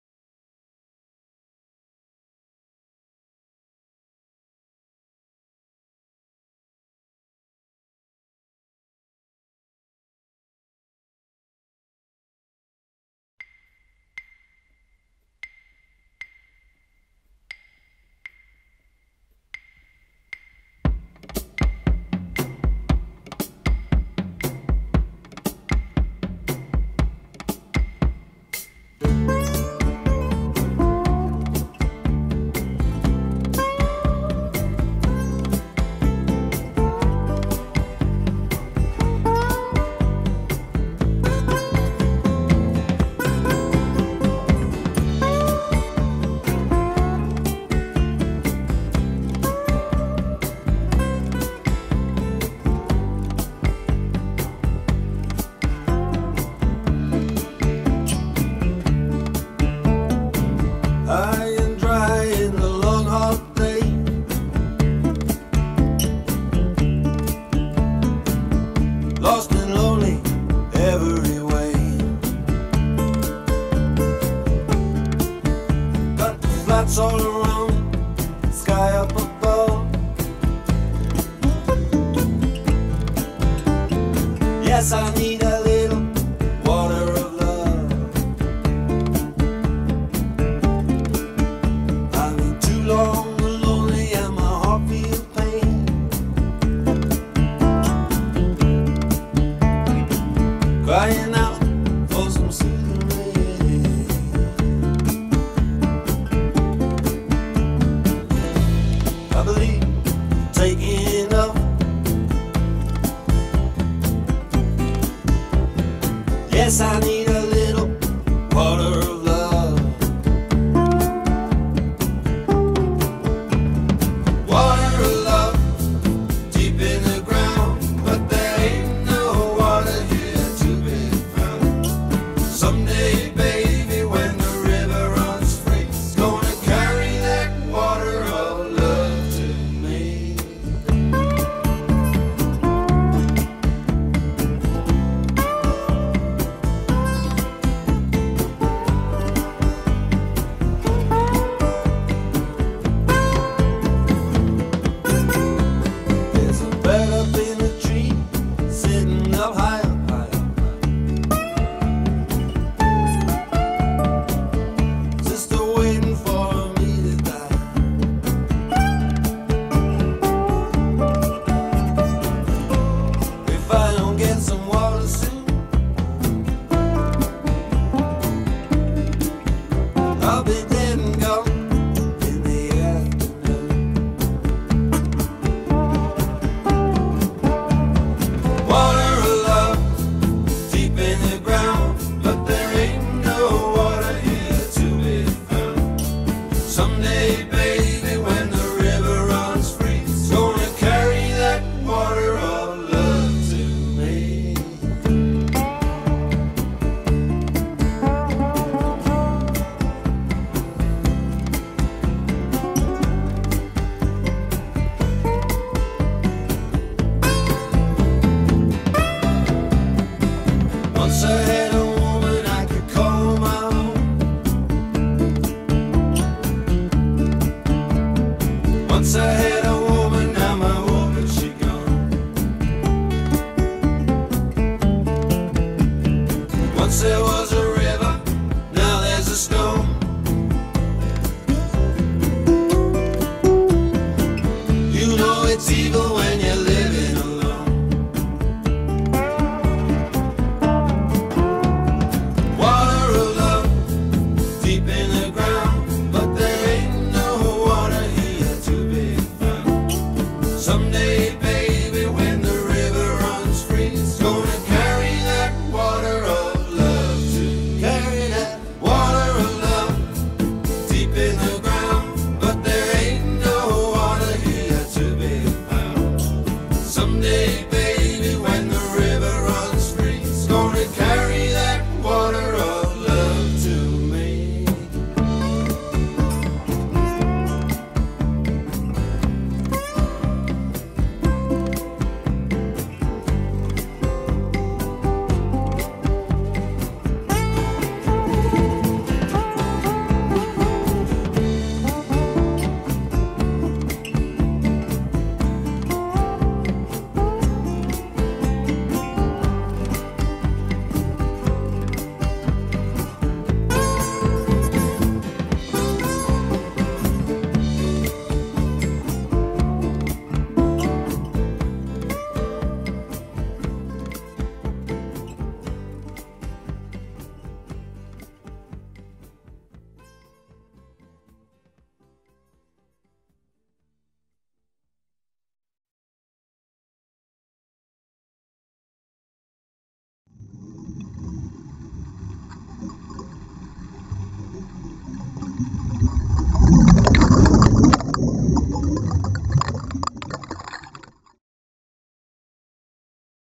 Diving Palau 2017
I went to Palau in 2017 on a dive travel trip with Island Divers. What an amazing trip! Palau offers something for every type of recreational diver. Wrecks, large pelagics, frequent spawning events, caverns, lots of hard and soft corals, kayaking the beauty of the Rock Islands, WWI and WWII history, and excellent land excursions to name a few things.